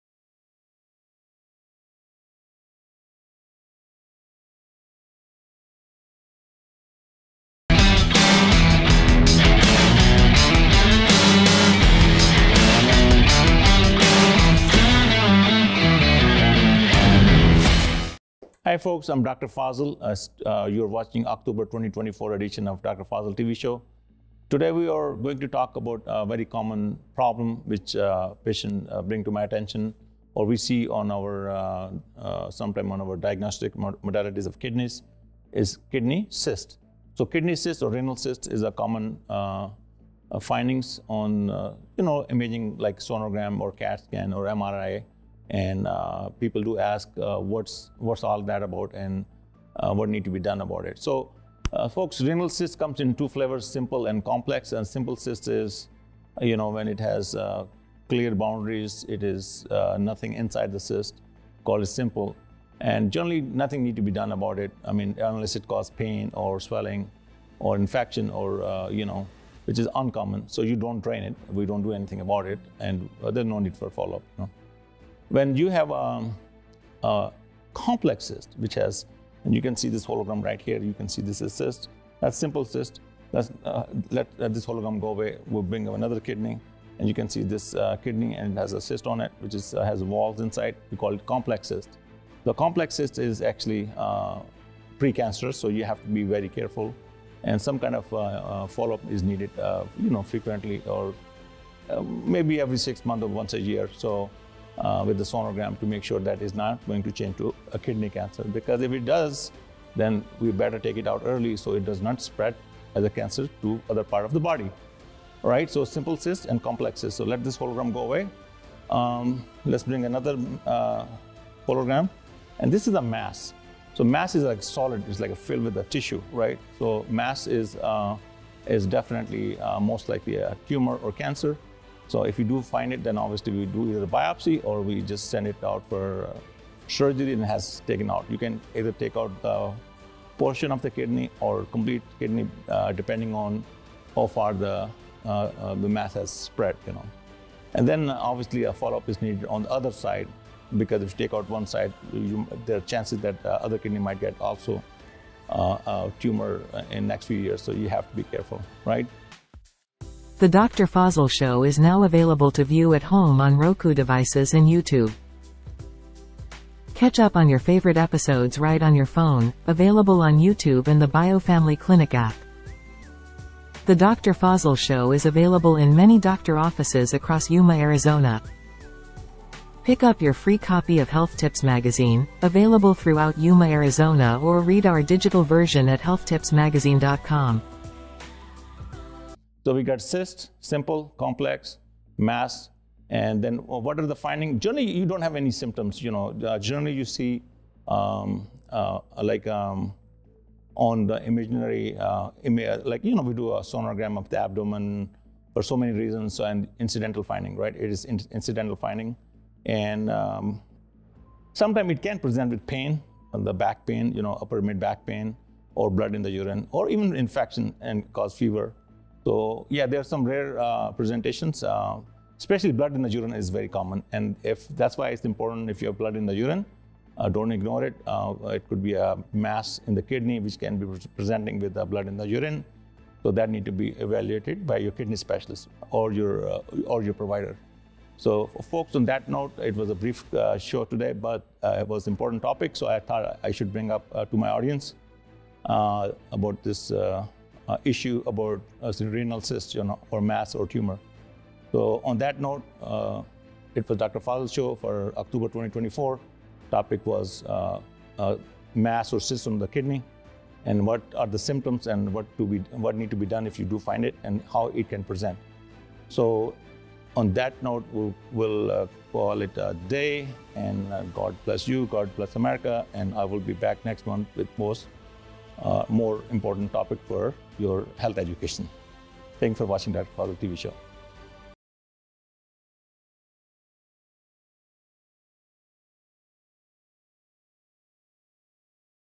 Kidney cysts, don't ignore the symptoms
Kidney cysts might be a pretty common occurrence in most people, but how do you know if it's a bad thing. If not taken care of with early intervention it could turn deadly. From a small simple cyst to a mass the could be a cancerous tumor, Dr. Fazil covers everything in-between to make sure you get the correct diagnosis and treatment early enough by recognizing the symptoms and what it could be.
All music and images provided and licensed by Shutterstock.